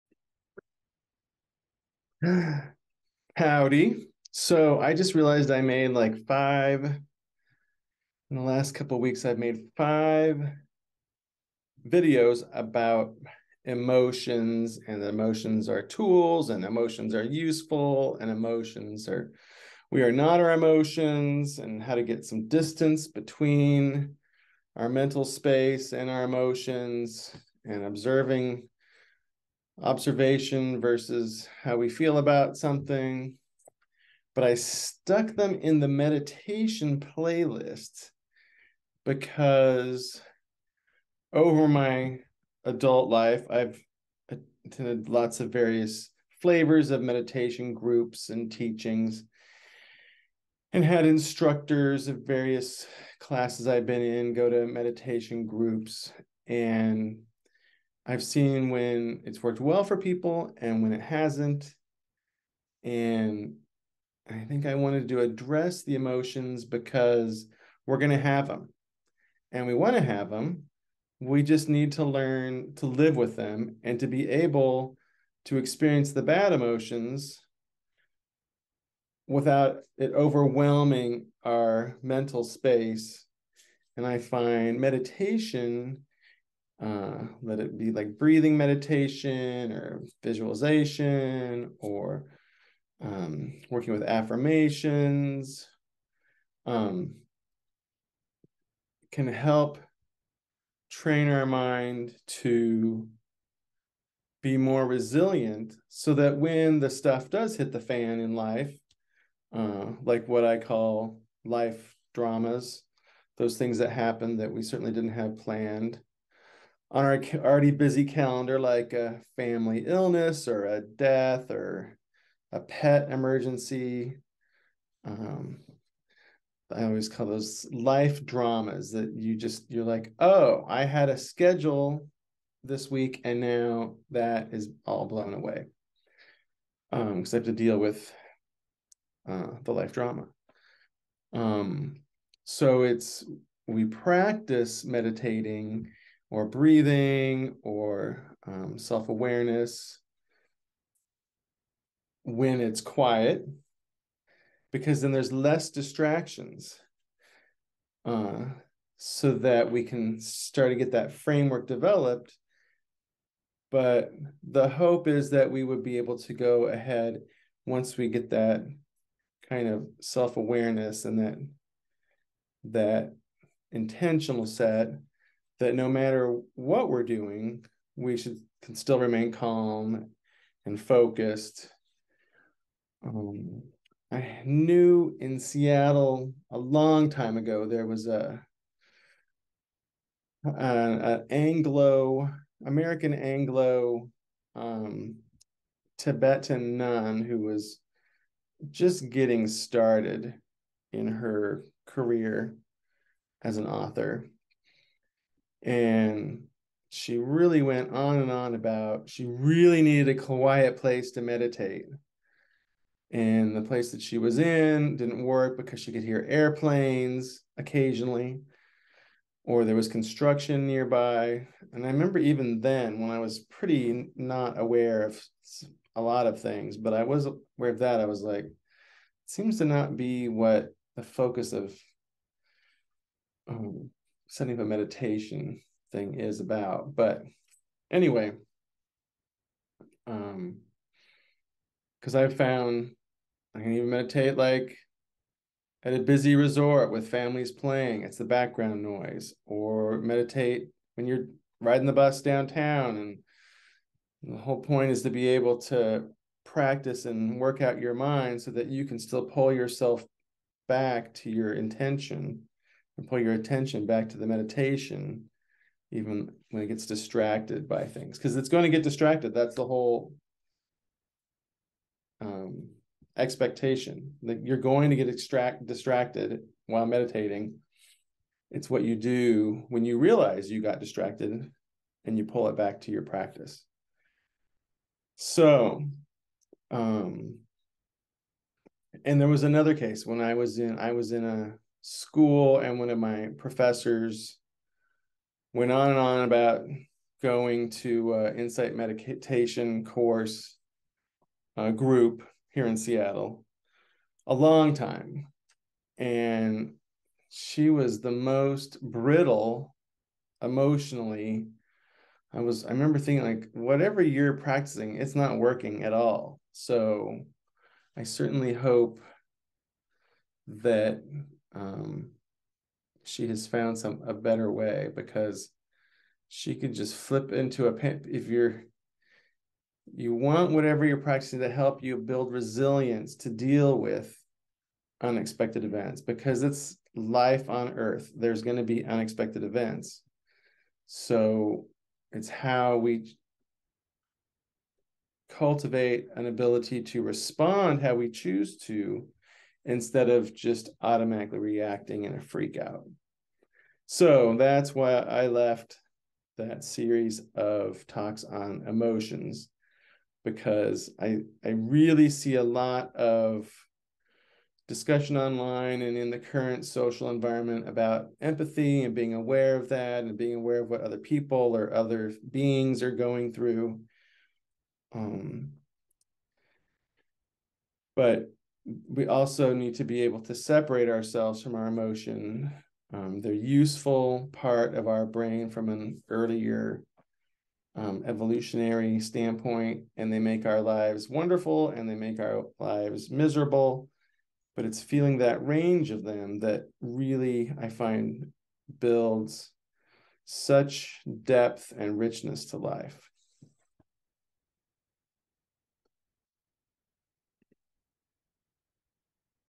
7 Life drama will happen how will you respond
Life drama will happen. Don't needlessly create more.  Choose how you will respond instead of reacting by reflex.